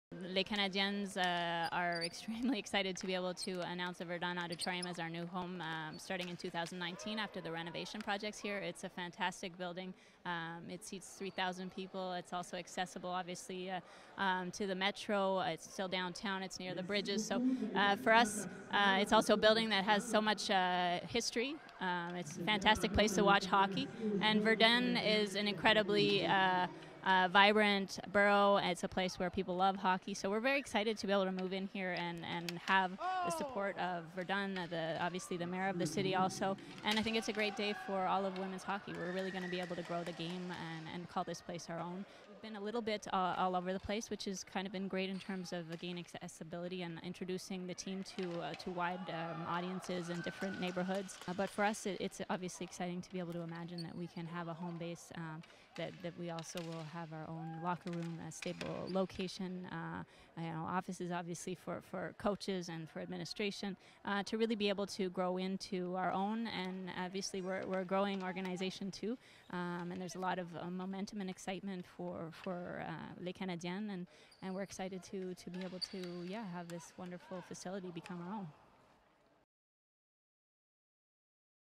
Les Canadiennes de Montreal get a permanent home
Players from Les Canadiennes de Montreal play a ball hockey game against students from Notre Dame des Sept Douleurs school following announcement Monday September 18, 2017 that the team will move permanently into the Verdun Auditorium in 2019.  The team, which competes in the Canadian Women's Hockey League, has played in several arenas for the last few seasons. (John Mahoney /MONTREAL GAZETTE)